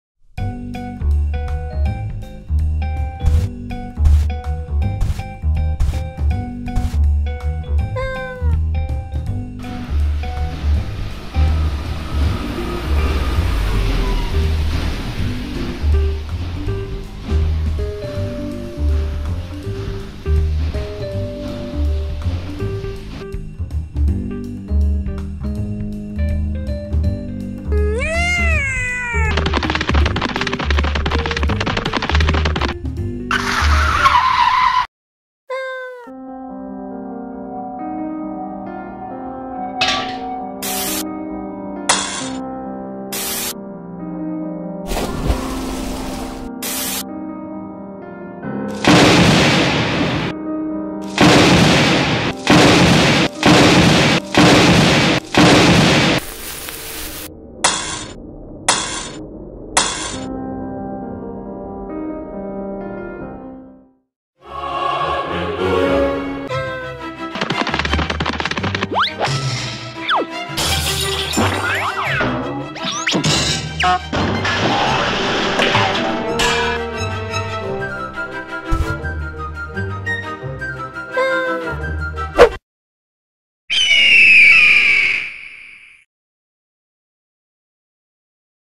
Last Life
A little animation I've done for my Animation class in HS. It's about a cat who's already lost 8 out of his 9 lives and is willing to risk it all in a daaangerous factory for TUNA!